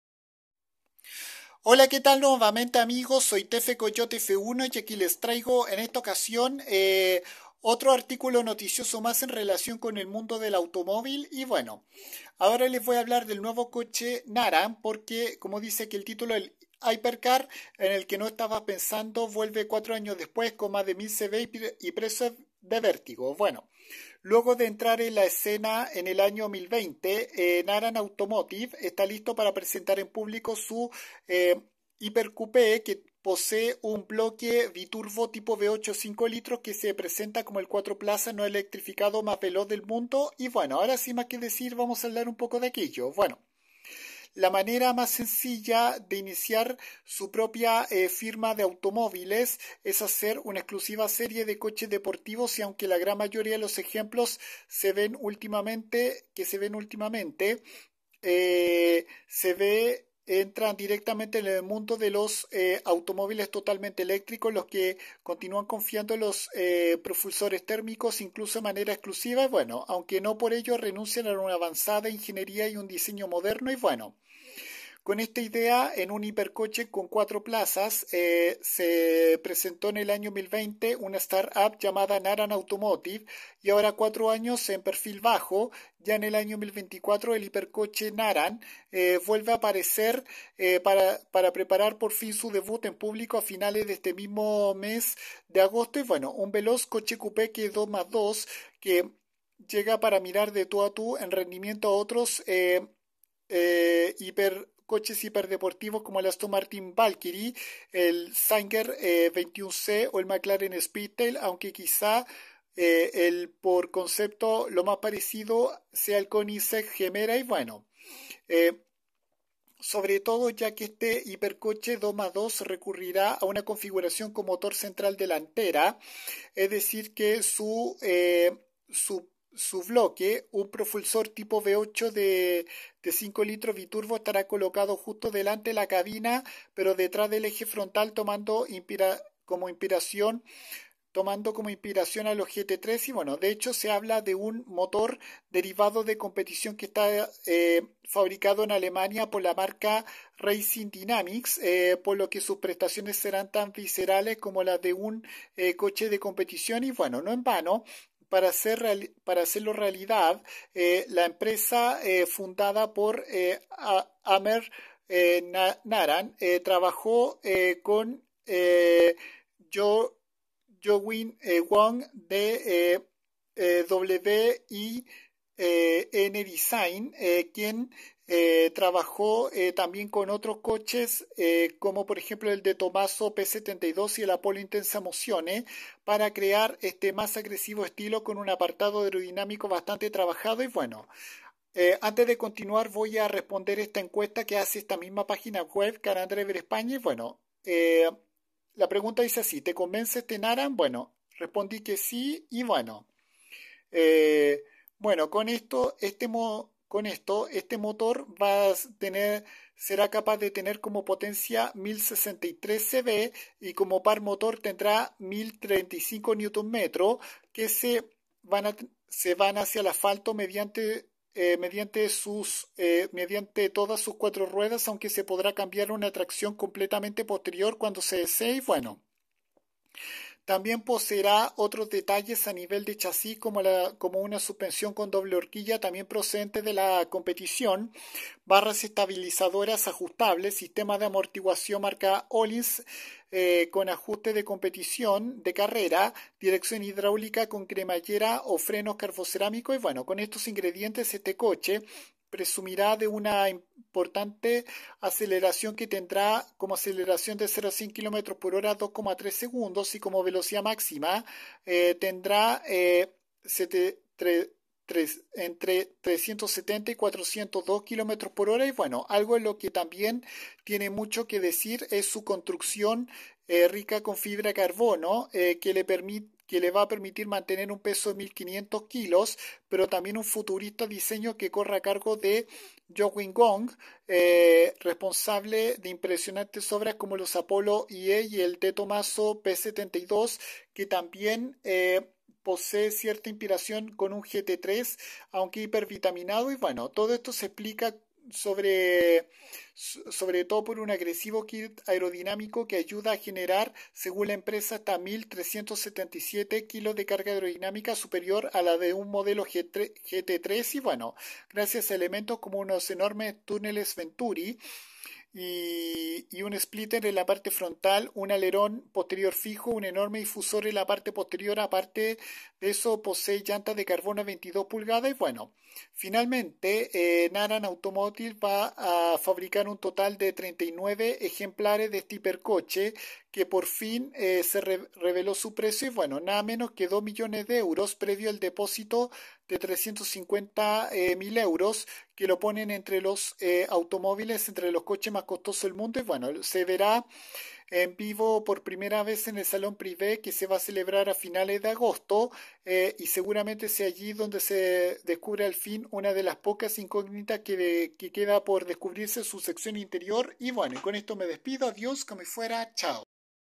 Naran Automotive información
Esta nueva marca automotriz presentará próximamente su nuevo hypercar 2+2 luego de que fuera presentada de manera oficial 4 años antes en 2020 y será un coche con motor biturbo tipo V8 de 5.0 litros